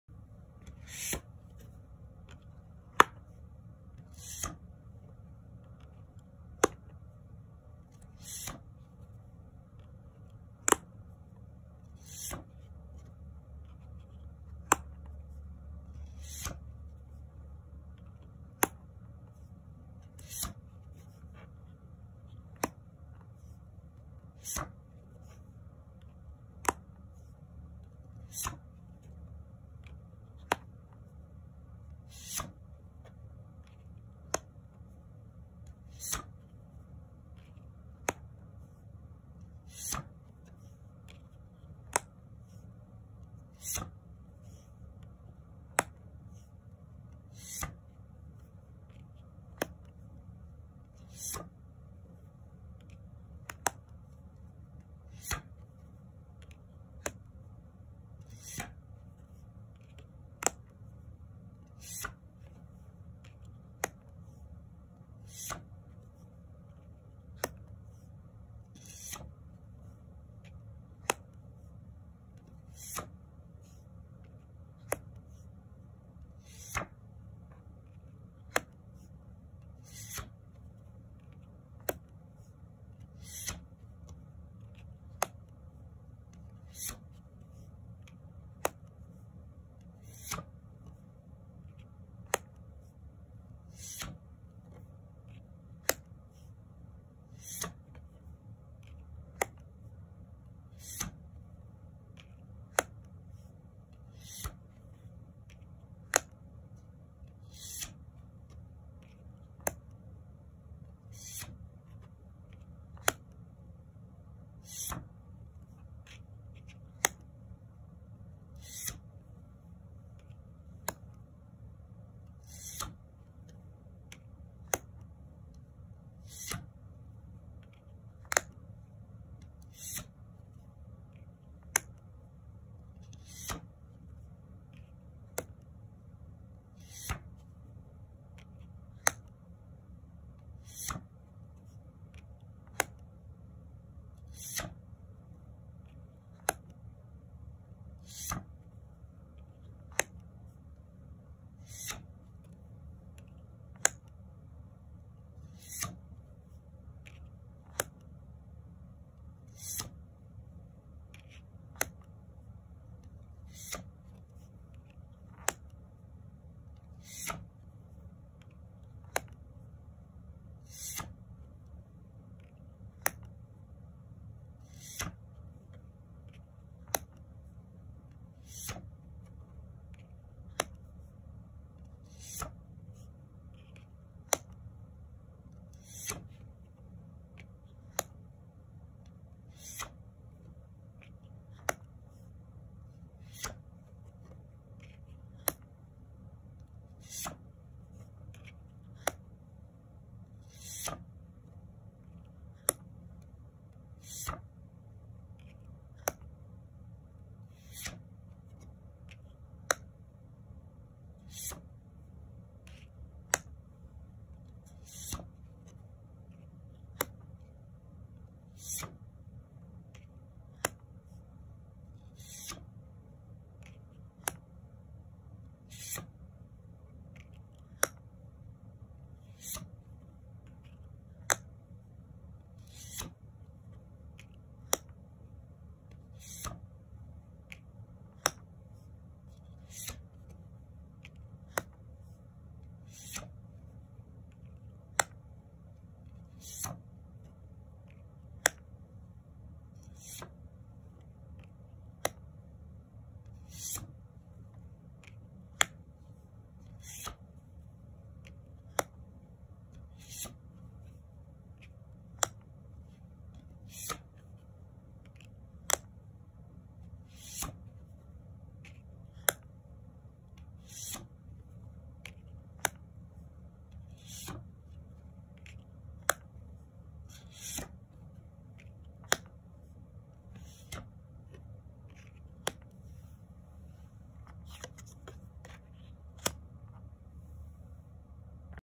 Erotic Fantasy Tarot Quick Flip
A quick flip through of Eon & Simona Rossi's deck, Erotic Fantasy Tarot, published by Lo Scarabeo.